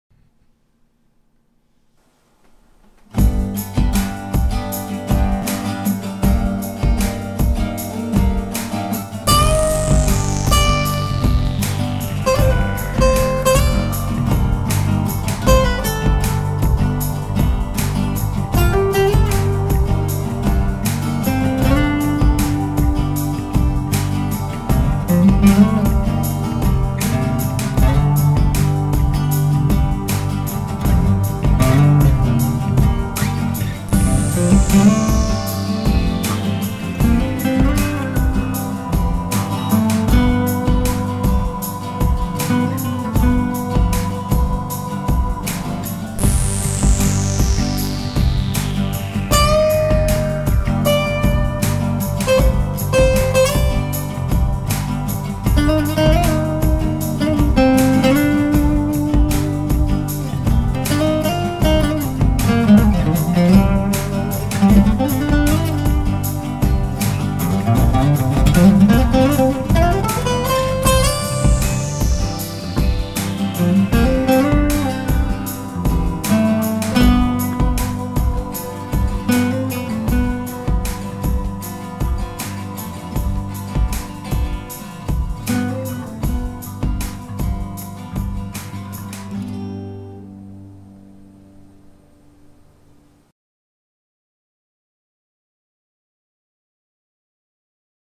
Acoustic Instrumental 2011
This is a small piece of music I wrote for a class video project in 2011.